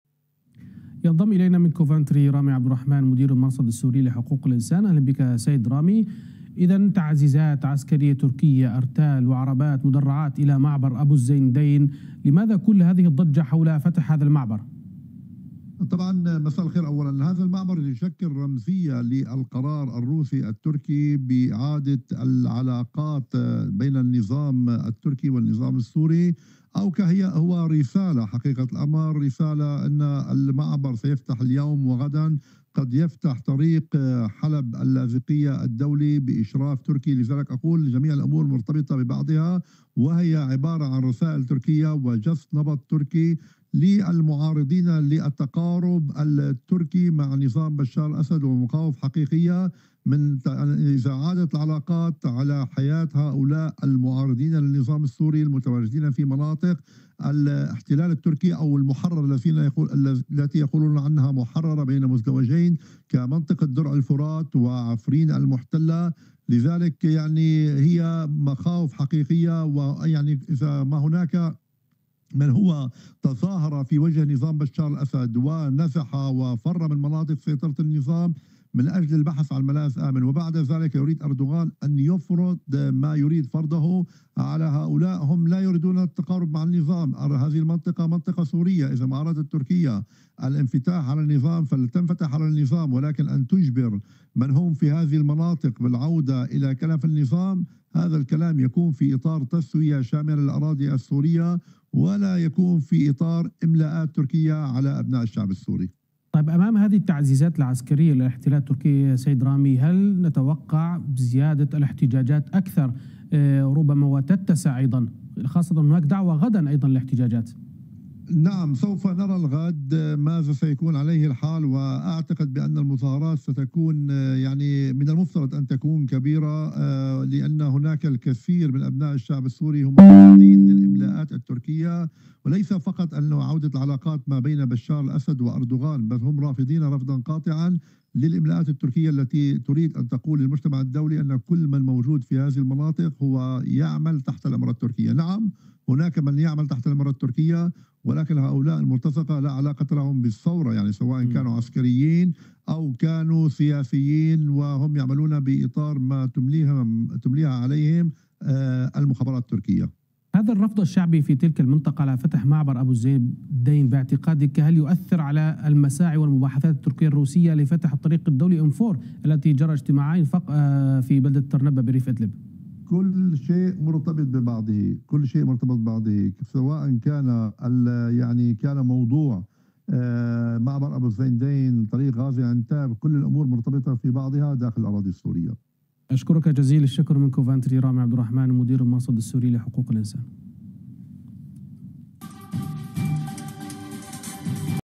إعادة افتتاح معبر أبو الزندين يشكل رمزية للجهود الروسية-التركية في إعادة العلاقات بين أنقرة ودمشق..
مدير المرصد السوري: إعادة افتتاح معبر أبو الزندين يشكل رمزية للجهود الروسية-التركية في إعادة العلاقات بين أنقرة ودمشق.. وهي رسالة لجس النبض وللمضي قدما بافتتاح طريق حلب اللاذقية m4 لاحقا.. أردوغان يريد أن يفرض على المعارضين للنظام السياسة التركية وإعادة العلاقات والانفتاح على النظام وهذا ينطبق على "المرتزقة" سواء مدنيين أو عسكريين.. والمحتجون على افتتاح المعبر لا يمكن أن يتقبلوا الاملاءات التركية على قرارهم ومصيرهم ومصير أبناء الشعب السوري.
والرفض الشعبي على افتتاح المعبر سيؤثر سلبا على إعادة افتتاح طريق "m4" وطريق غازي عنتاب -حلب لاحقا